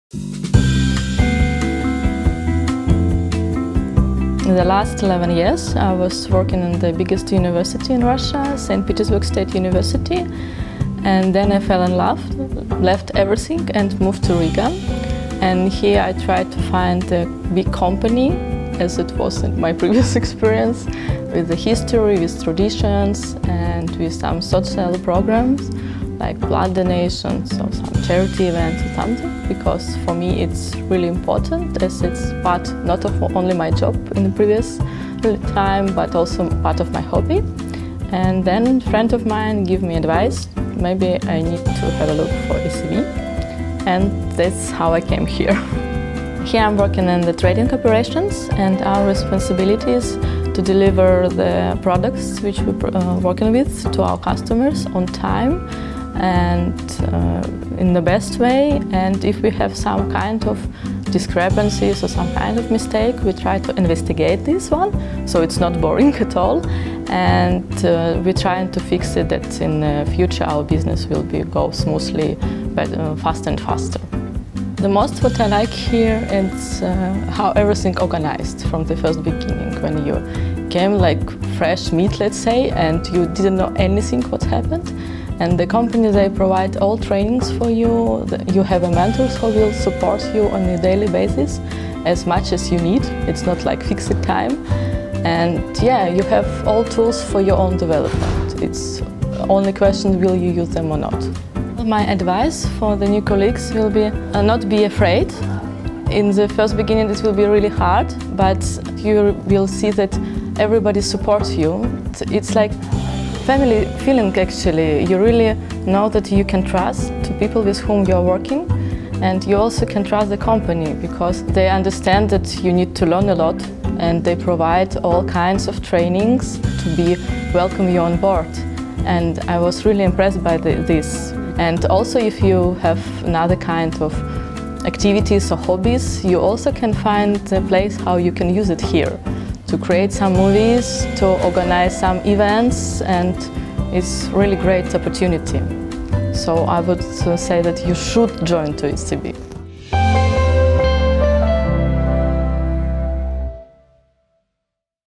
New experience in new country – work in SEB Global Services Riga
Marina Smirnova moved to Riga from Russia where she previously worked in Saint Petersburg University. She looked for company with history, traditions and social program. So SEB Global Services Riga was logical choice.